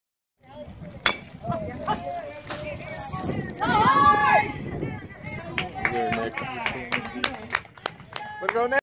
6-9-17 - Nick Beltran legs out an infield hit
Eaton at Fort Morgan Mustangs 15 and under baseball in game 1 of a doubleheader on June 9, 2017. Eaton defeated Fort Morgan, 12-5.